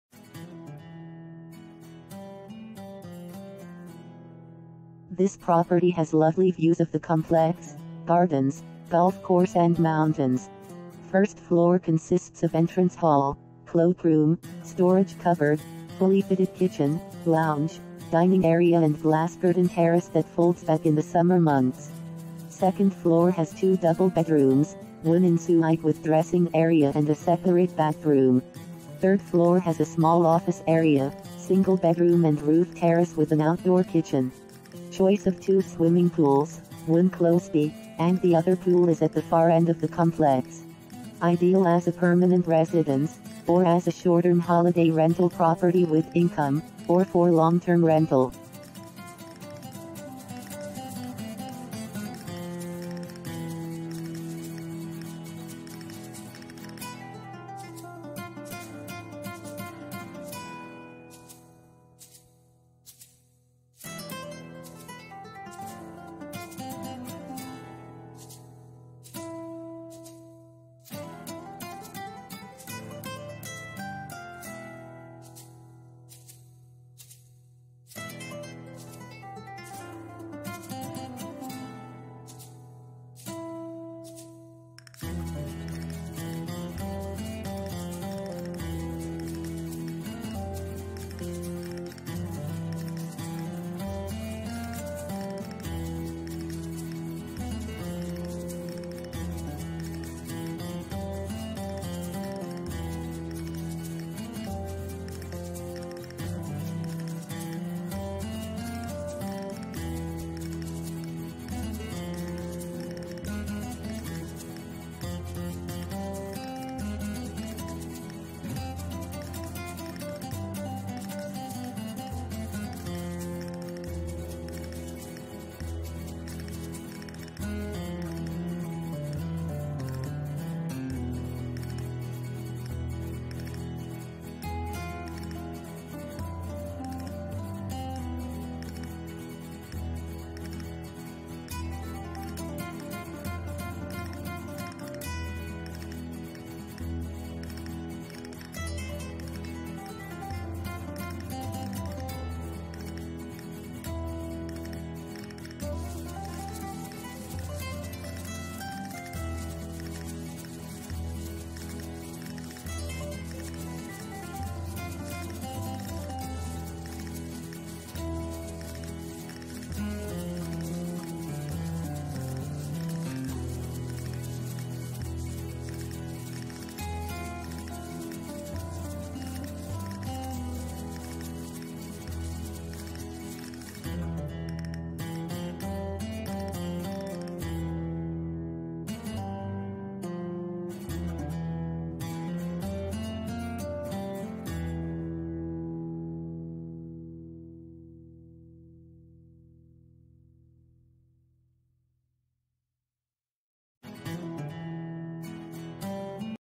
3 Bedrooms Townhouse in Alhaurin Golf, Costa Del Sol FOR SALE &euro; 219,000
3 Bedrooms Townhouse in Alhaurin Golf, FOR SALE &euro; 219,000
This property has lovely views of the complex, gardens, golf course and mountains.
First floor consists of entrance hall, cloakroom, storage cupboard, fully-fitted kitchen, lounge, dining area and glass-curtain terrace that folds back in the summer months.
Second floor has two double bedrooms, one en-suite with dressing area and a separate bathroom.
Third floor has a small office area, single bedroom and roof terrace with an outdoor kitchen.
Choice of two swimming pools, one close-by, and the other pool is at the far end of the complex.
Ideal as a permanent residence, or as a short-term holiday rental property with income, or for long-term rental.
Covered Terrace;Fitted Wardrobes;Satellite TV;Storage Room;Ensuite Bathroom;Restaurant On Site
Frontline Golf;Close To Town;Close To Schools